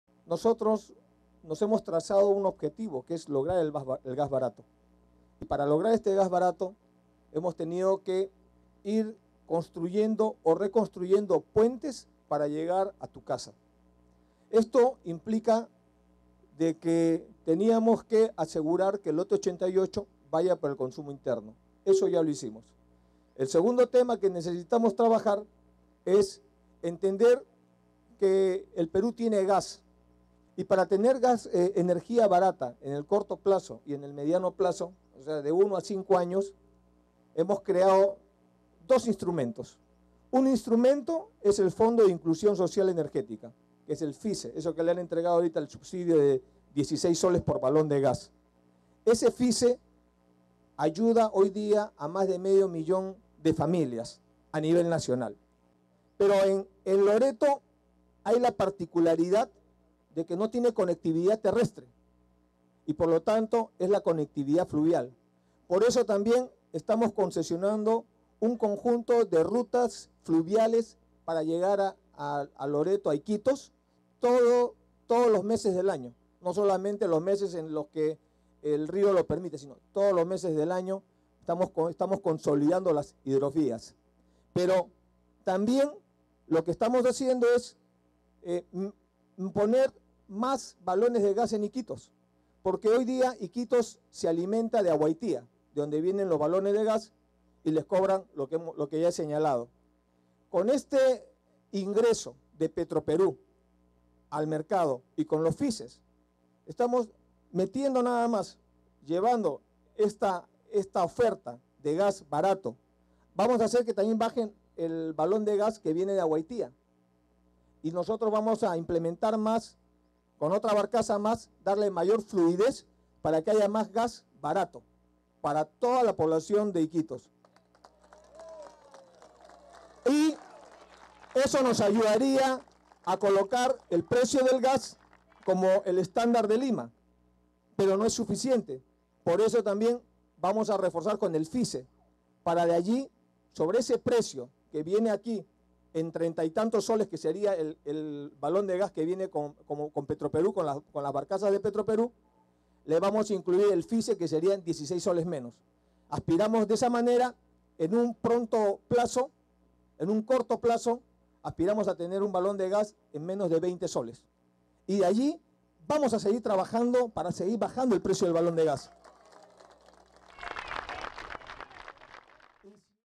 En Iquitos se inicia promesa de gas barato
Presidente de la República, Ollanta Humala Tasso inauguró el servicio de transporte fluvial de gas licuado de petróleo (GLP) desde el Puerto de Yurimaguas al Puerto de Iquitos.